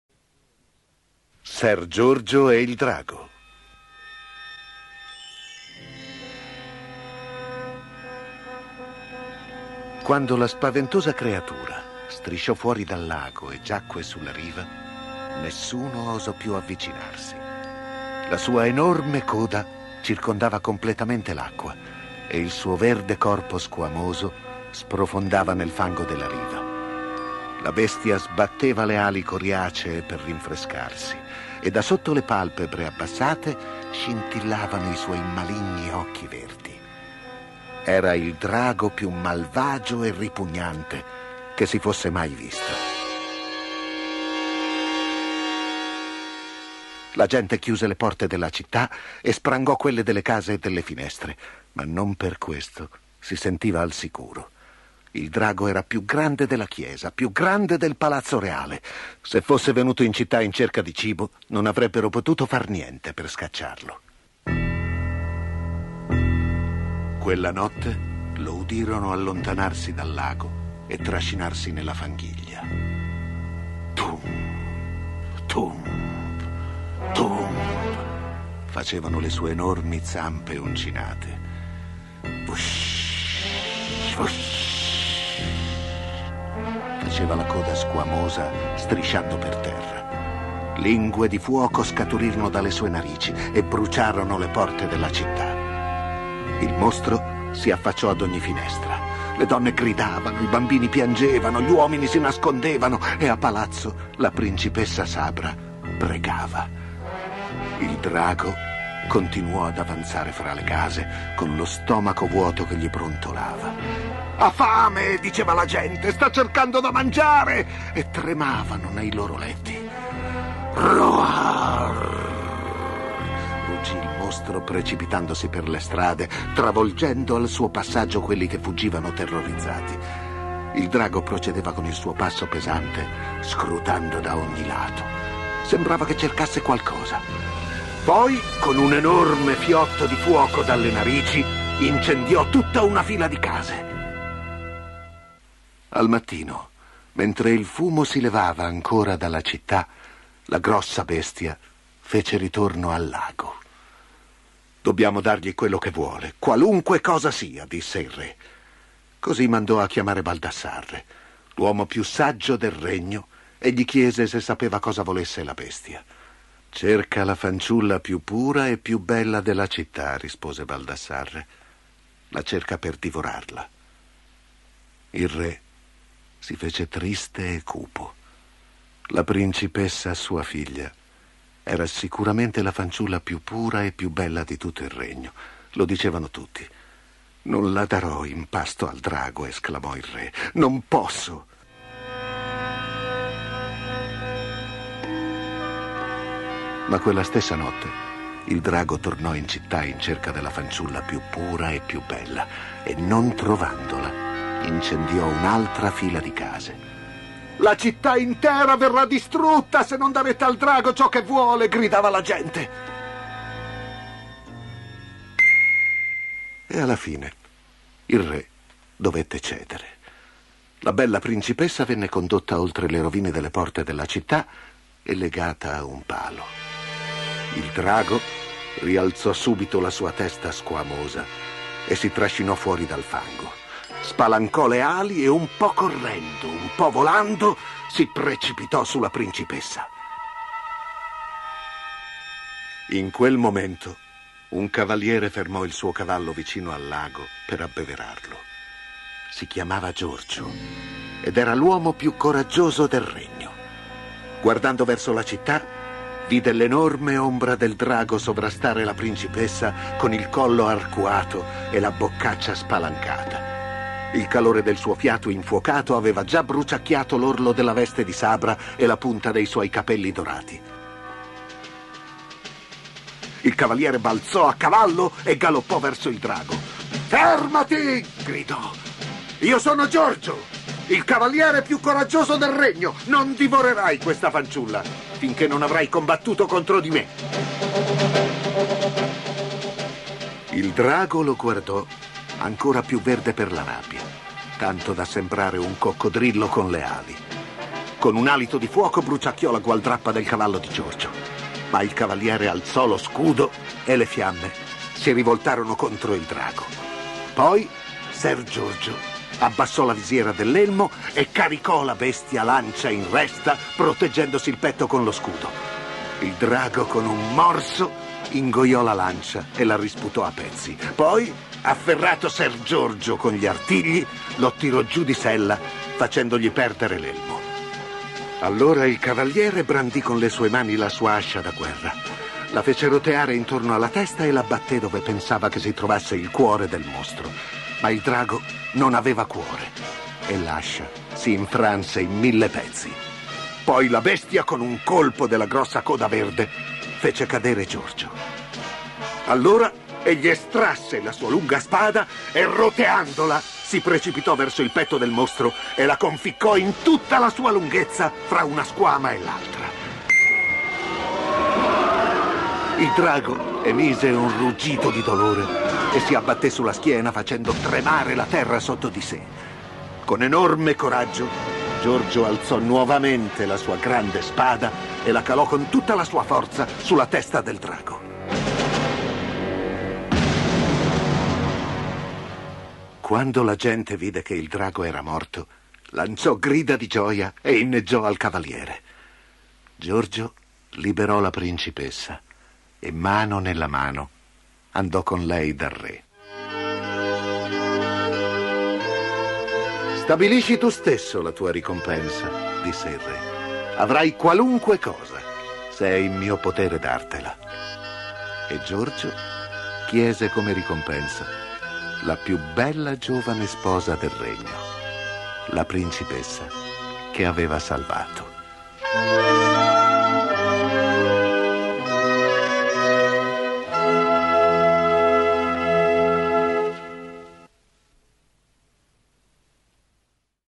Ser Giorgio e il drago
Una storia di castelli, draghi e principesse. nel più classico dei mondi misteriosi e incantati!